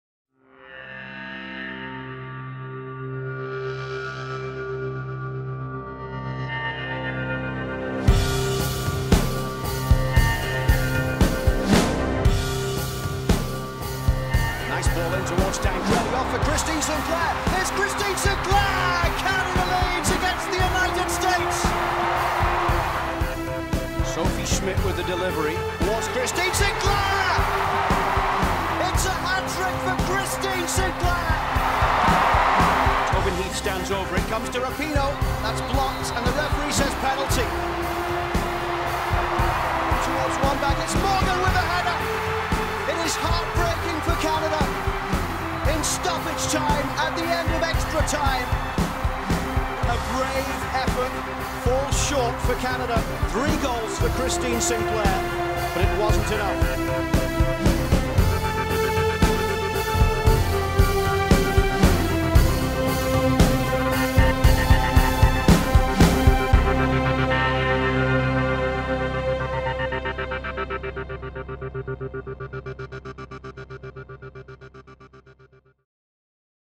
Canada v USA, 'The Rematch', June 2nd at BMO Field
Following the Olympic semi-final that has been dubbed by many as the 'greatest women's football match of all time' - North American rivals Canada and USA are set to renew hostilities at BMO Field in Toronto on June 2nd.

Complete match info can be found at:
CanadaSoccer.com